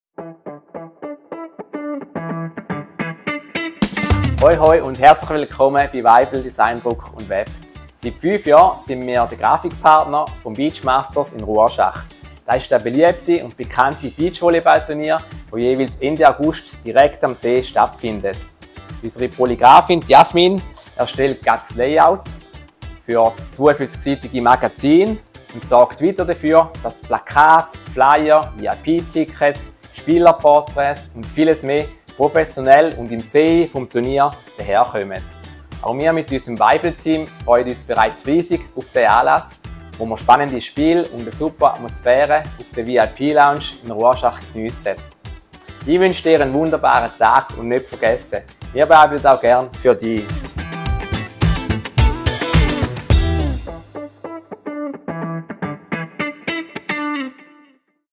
Frische Grafikdesigns fürs Beachvolleyball-Turnier in Rorschach
Seit über fünf Jahren sind wir Grafikpartner vom Beachmasters in Rorschach. Das ist das beliebte und bekannte Beachvolleball-Turnier, das jeweils Ende August direkt am See stattfindet.

Unsere Polygrafin Jasmin erstellt gerade das Layout und bearbeitet die Bilder für das 52-seitige Magazin und sorgt weiter dafür, dass Plakate, Flyer, VIP-Tickets, Spielerportraits und vieles mehr professionell und im CI des Turniers daherkommen.

Auch wir mit unserem Weibel-Team freuen uns bereits riesig auf diesen Anlass, wo wir spannende Spiele und eine super Atmosphäre auf der VIP-Lounge in Rorschach geniessen.

Wir weibeln für deinen Erfolg!

Weibel Druck & Design AG
Grafik | Druck | Web | Film

«Wir wollen die beste Druckerei der Schweiz werden!»
Dani Weibel, Inhaber